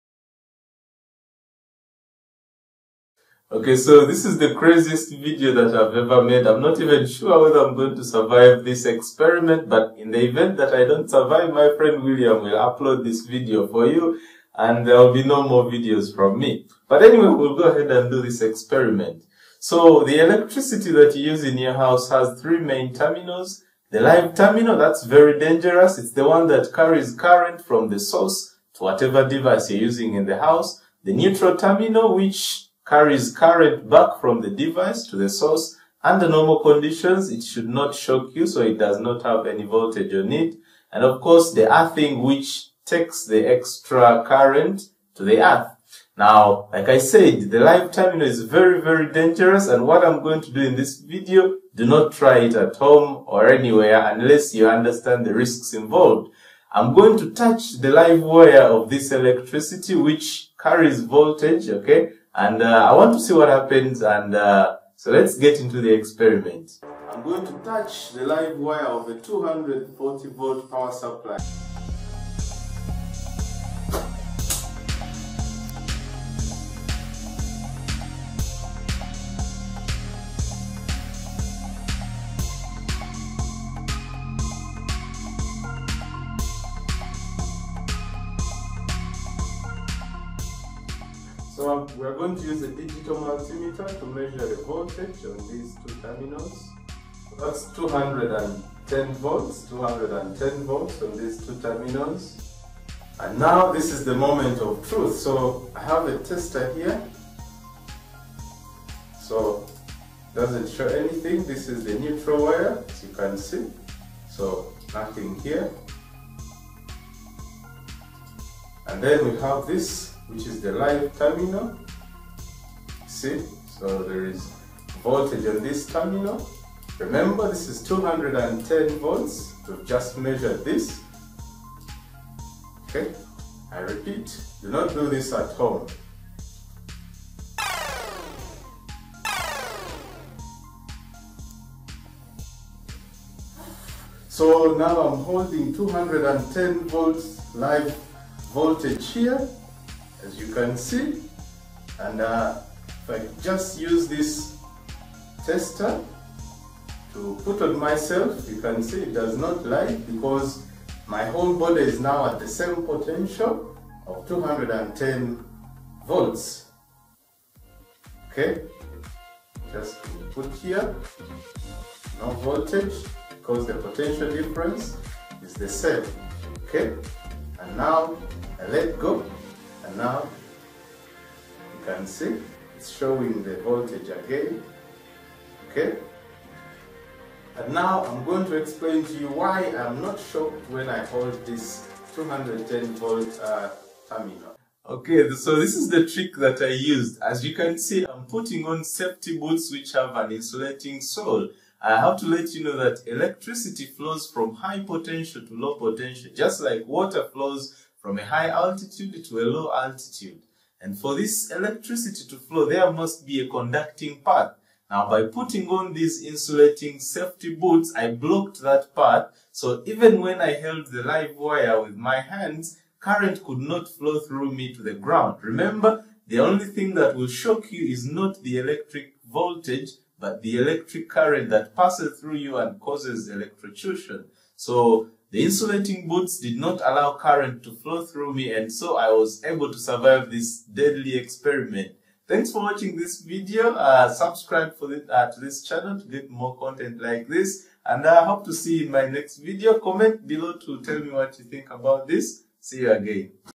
I touched a LIVE WIRE of Electricity - Do no try this !!
In this video, i try to show that electrocution is caused by current flowing though the body and not just the magnitude of voltage. i did that by holding a LIVE wire of electricity while wearing safely insulated boots.